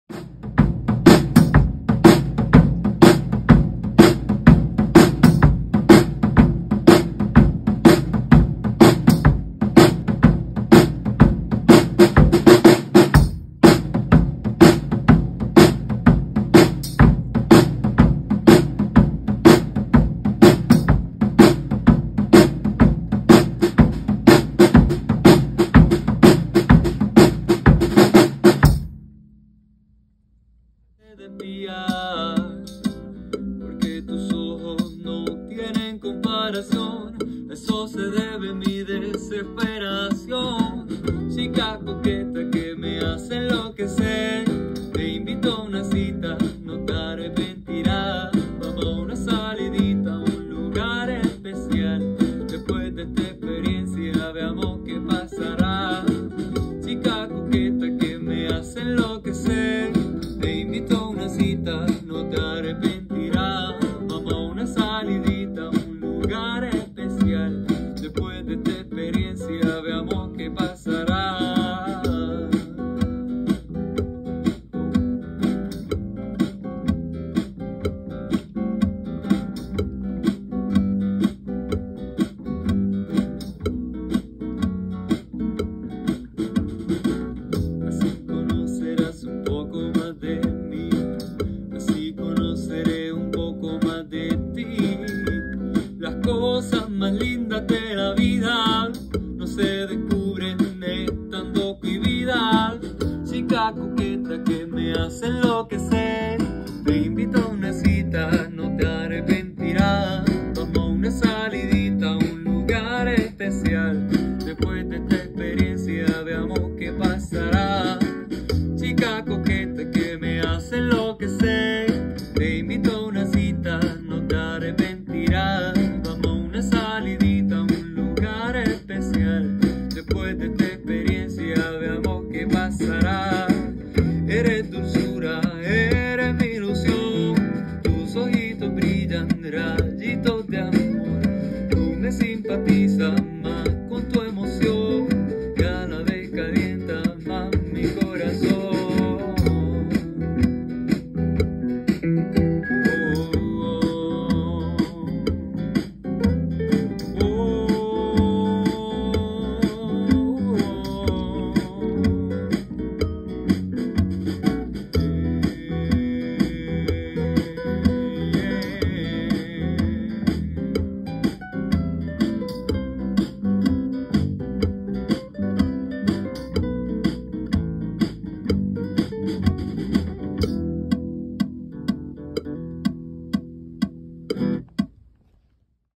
Tracking Drums@Sagrado-Jorge Flynn
Grabación en los cursos de Producción Musical: MUS 315 & 325 para las becas de nuestros estudiantes participantes para el ASCAP Foundation. EADIC (Música) plantando bandera. Agradecido siempre del amigo, colega y miembro de nuestra facultad Josiane Feliciano por el apoyo. Seguimos provocando espacios de transformación. 🇵🇷🇵🇷🇵🇷🔥🔥🔥😎😎😎💪💪💪 musicaensagrado escueladeartessagrado sagradoedu leadership jorgeflynn goat Pearl Corporation